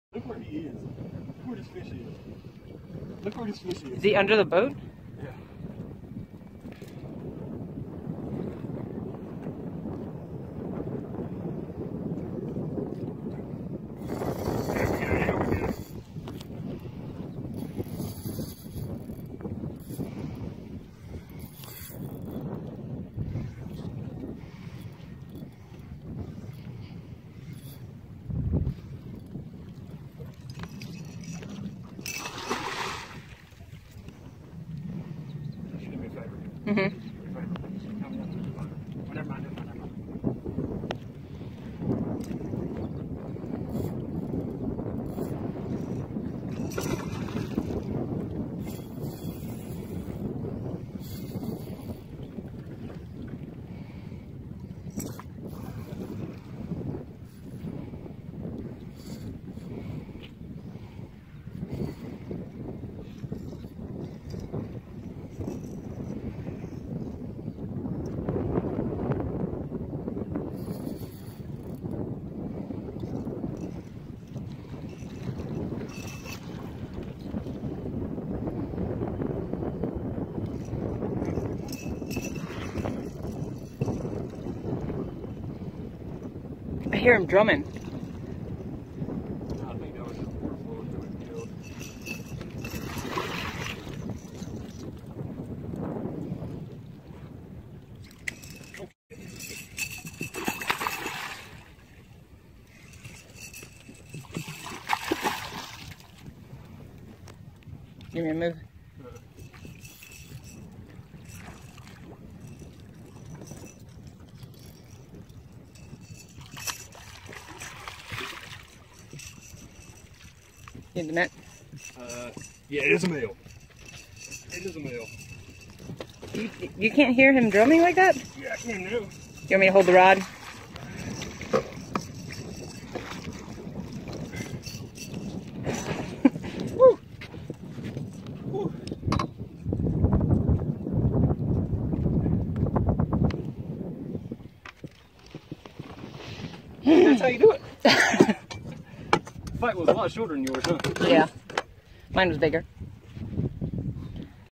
Pamlico Drumming 9-16-17
Just a typical Saturday morning on the Pamlico during big drum season. Fish was caught on a DOA Airhead under a Blabber Mouth Popping Cork.